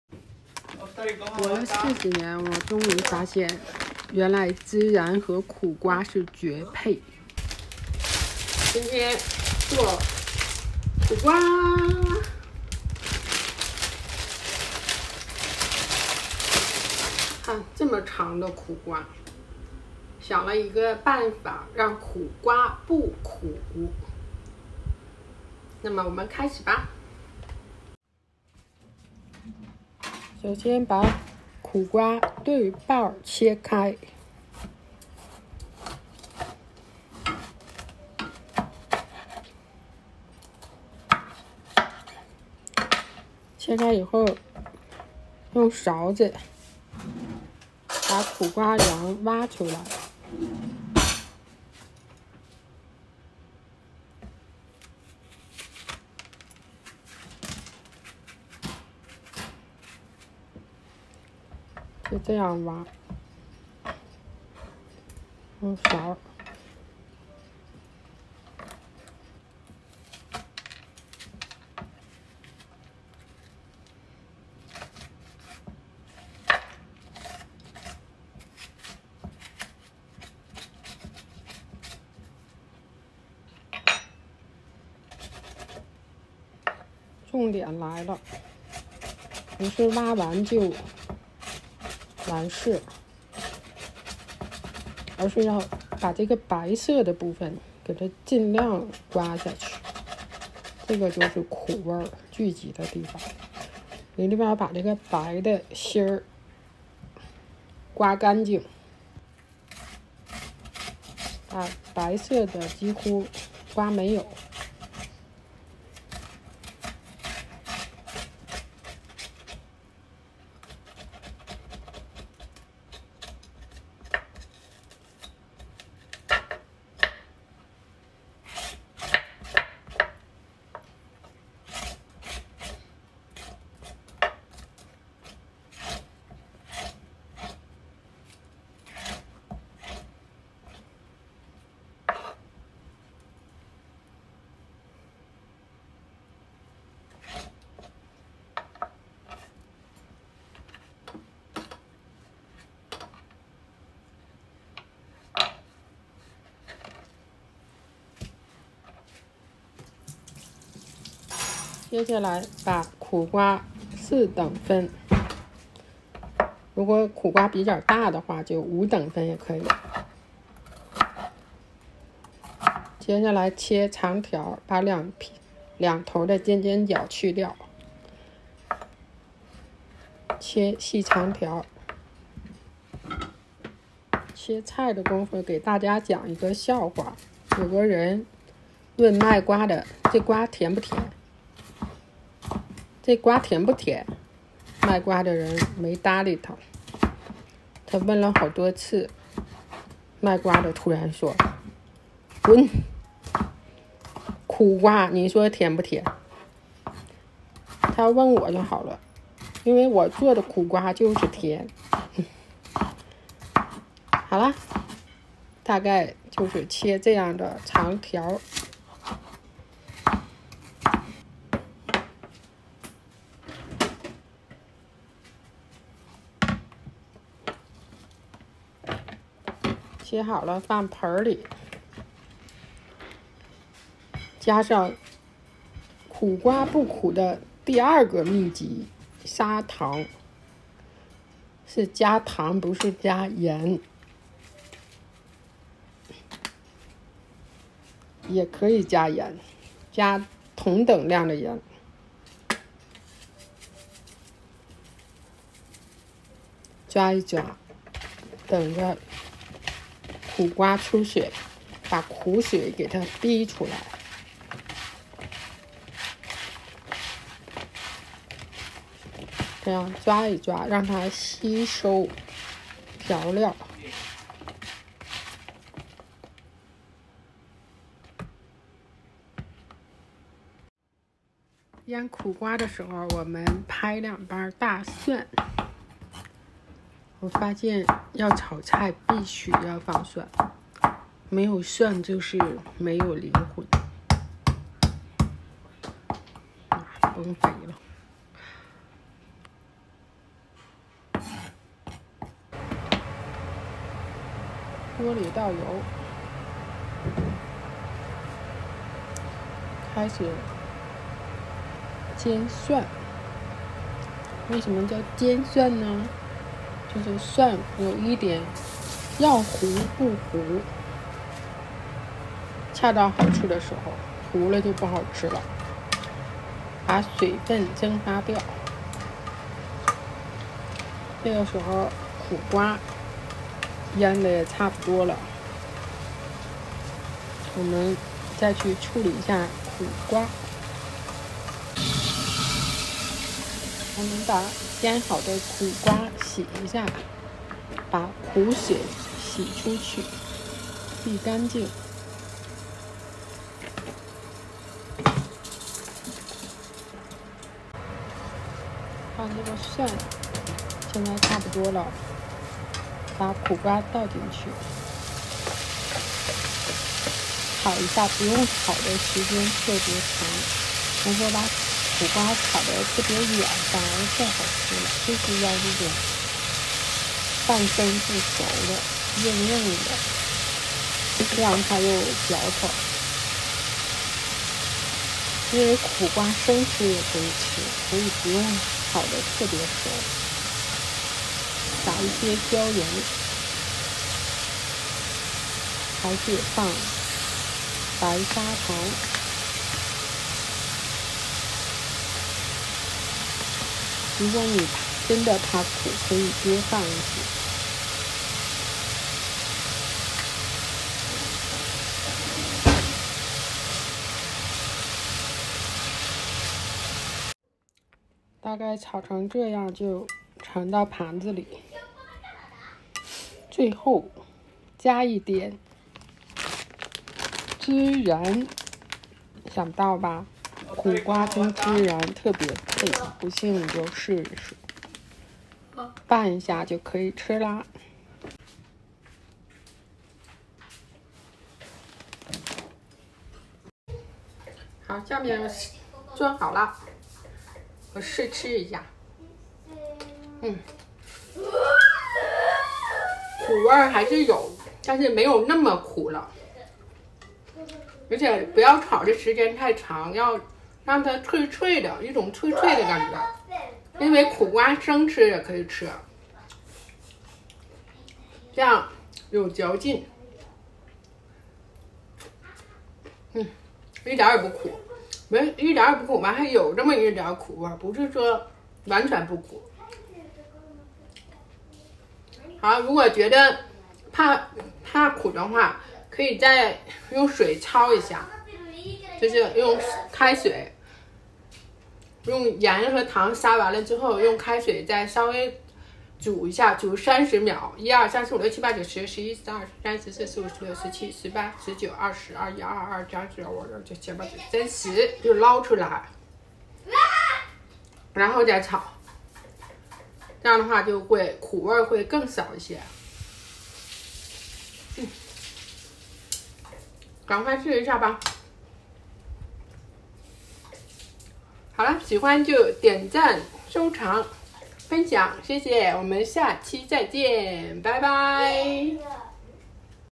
孜然和苦瓜的愛情
意外好吃😋
注意不要炒的時間過長
醃第一次放糖，炒的時候再放一次糖。
刮苦瓜的時候把白瓤刮乾淨。